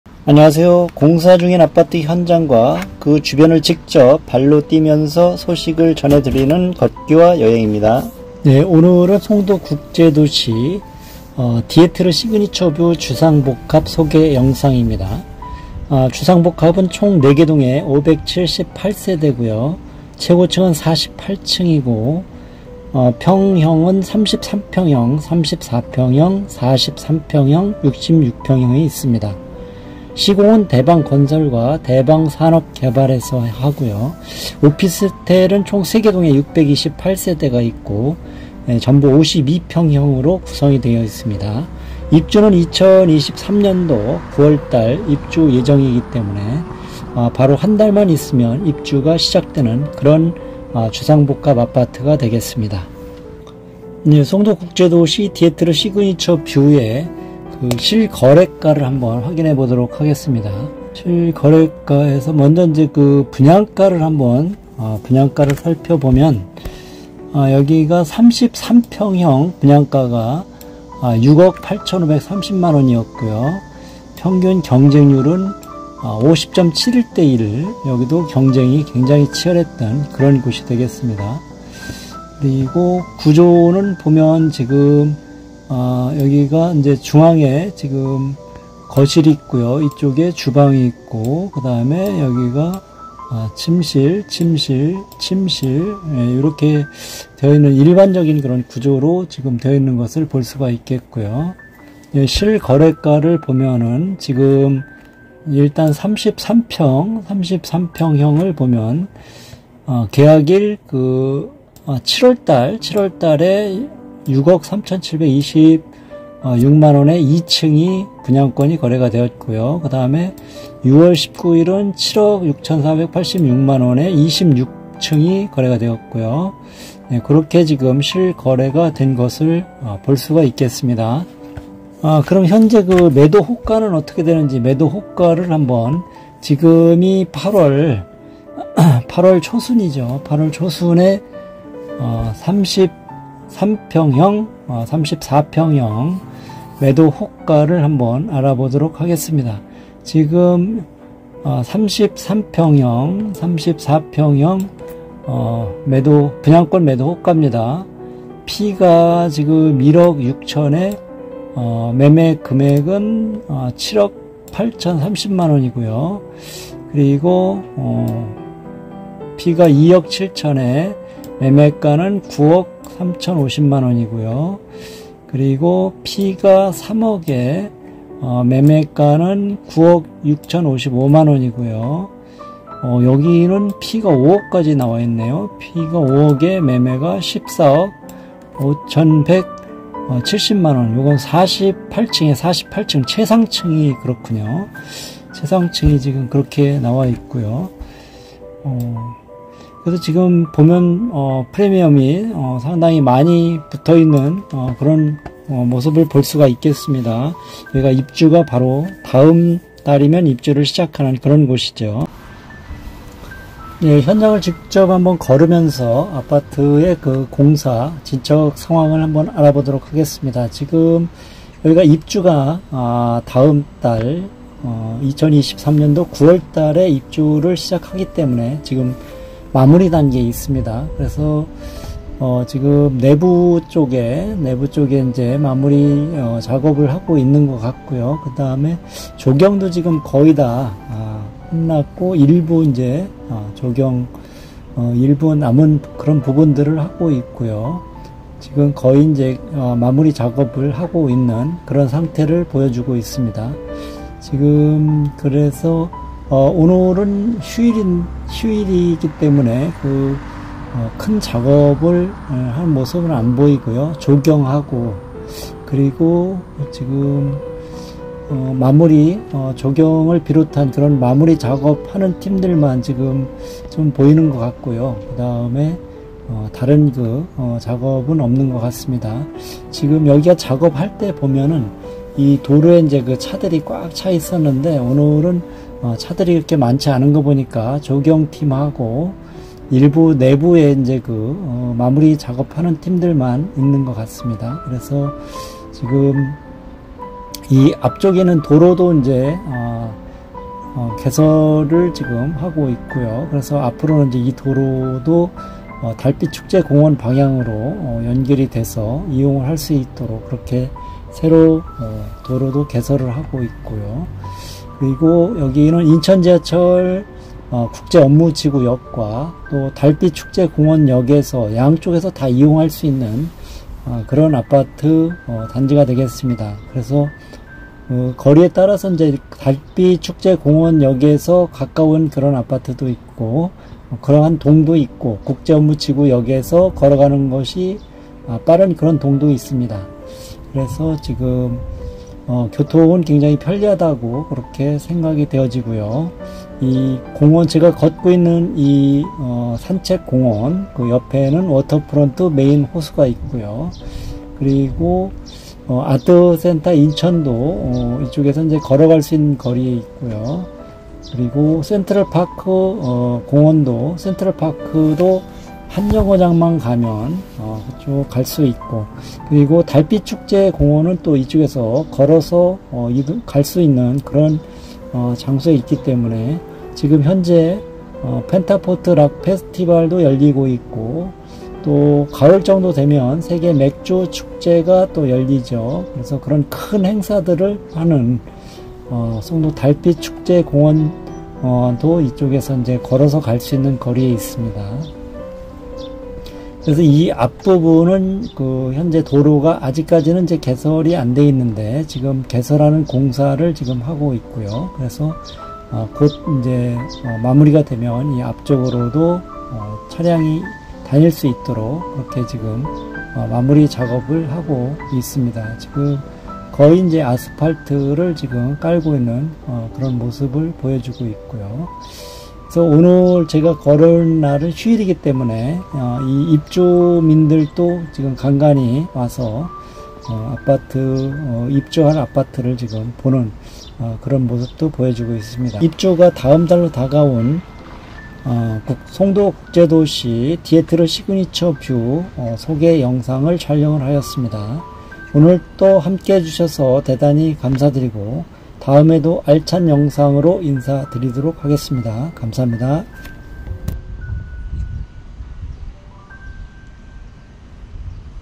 2023년 9월 입주예정 송도워터프런트 주변 송도국제도시디에트르시그니처뷰 아파트/송도워터프런트/송도달빛축제공원역/송도아파트/송도상승지역/송도주상복합/송도 신축아파트
안녕하세요!!!
이번 영상은 인천지하철 1호선 국제업무지구역과 송도달빛축제공원역  부근에 공사중인 송도국제도시디에트르시그니처뷰 아파트 소개 영상입니다.
아파트 4개동 578세대, 아파텔 3개동 628실, 판매시설 917실 지하3층에서 지상 48층까지 올라가는 아파트입니다.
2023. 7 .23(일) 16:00
날씨 : 흐림
온도 : 26℃